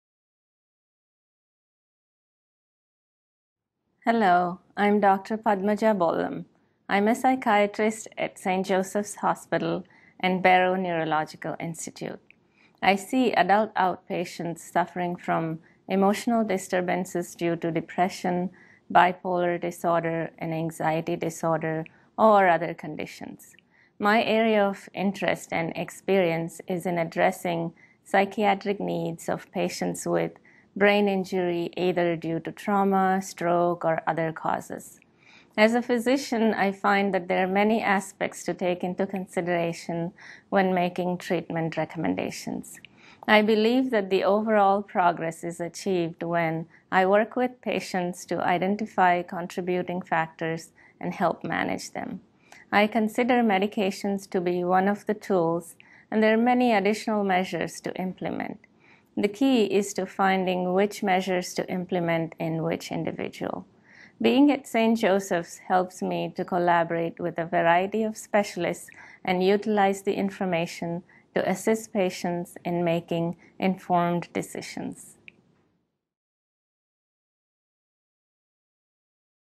Dr. Padmaja Bollam
Learn more about Dr. Padmaja Bollam, a psychiatrist at St. Joseph's Hospital and Medical Center in Phoenix, Arizona.